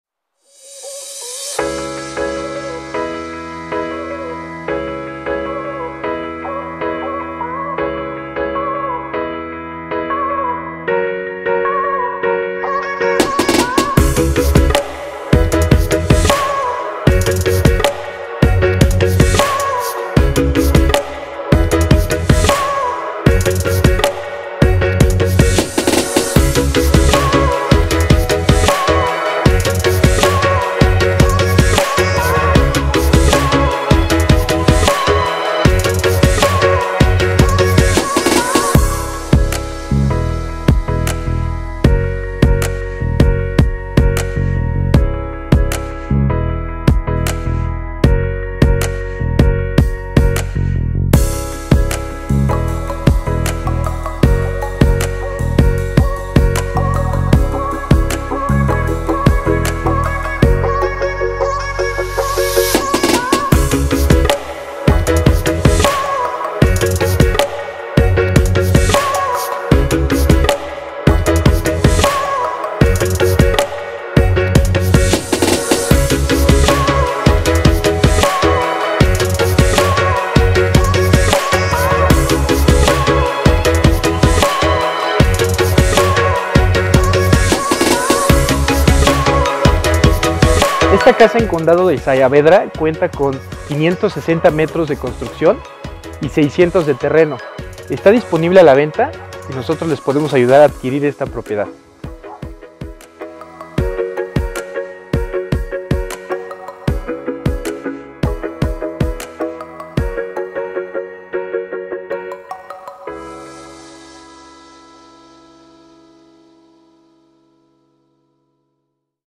La residencia de tus sueños en uno de los fraccionamientos más exclusivos
📍Fraccionamiento Condado de Sayavedra - Atizapán de Zaragoza
▶️ Una casa con estilo unico ubicada en una de las zonas más exclusivas del Estado de México

▶️ Seguridad y flitros de acceso 24/7

🛏 4 Recamaras
🚿 4.5 Baños
🚗 6 Estacionamientos

💲 16,000,000
✅ Aplica para crédito